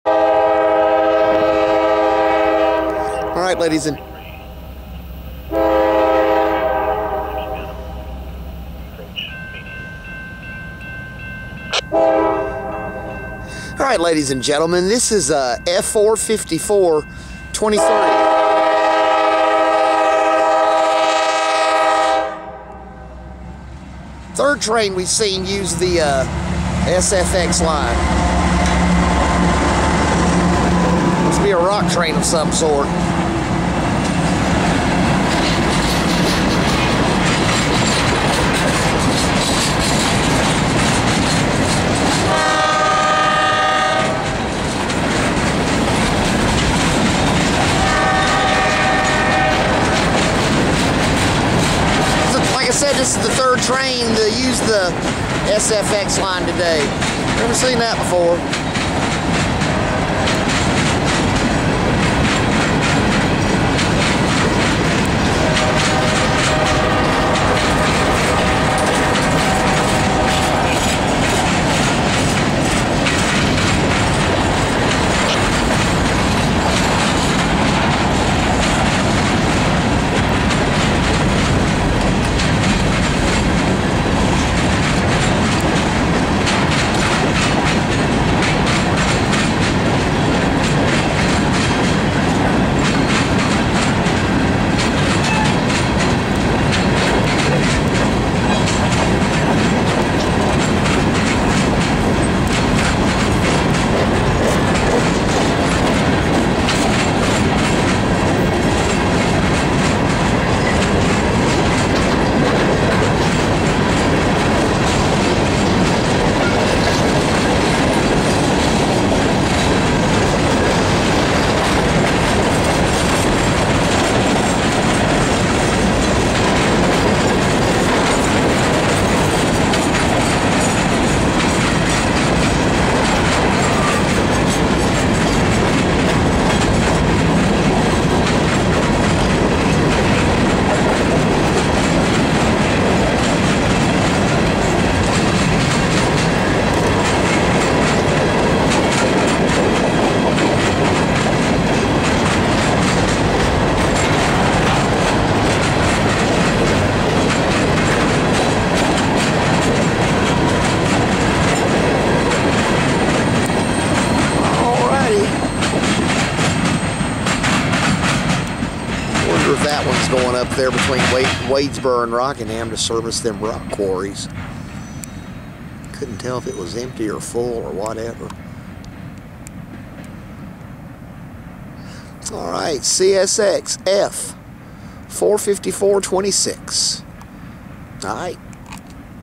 CSX F 454 23 Rock train 2 26 17 Hamlet Terminal SFX Line
Empty rock train CSX F 454 23 pulls southbound by Hamlet Crossing on it's way to the quarries between Rockingham and Wadesboro.  This would also be the 3rd train to use the East West Crossover track.